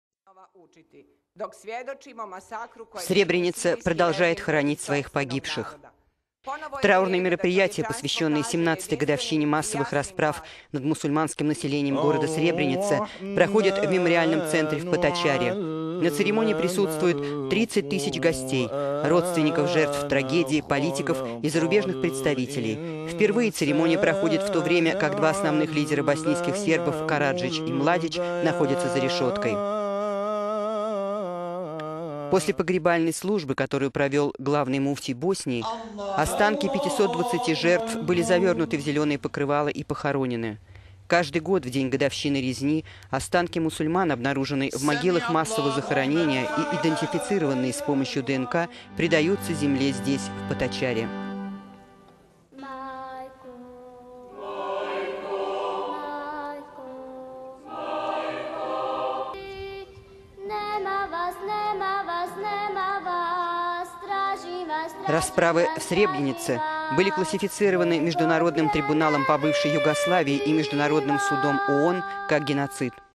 Сребренница продолжает хоронить погибших
Траурные мероприятия, посвященные 17-й годовщине массовых расправ над мусульманским населением города Сребреница, проходят в Мемориальном центре в Поточари.  На церемонии присутствуют 30 тысяч гостей - родственников жертв трагедии, политиков и зарубежных представителей. Впервые церемония проходит
в то время, как два основных лидера боснийских сербов Караджич и Младич, находятся за решеткой.

После погребальной службы, которую провел главный муфтий Боснии, останки 520 жертв были завернуты в зеленые покрывала и похоронены. Каждый год, в день годовщины резни, останки мусульман, обнаруженные в могилах массового захоронения и идентифицированные с помощью ДНК, предаются земле здесь, в Поточари.

Расправы в Сребренице были классифицированы Международным трибуналом по бывшей Югославии и Международным судом ООН как геноцид.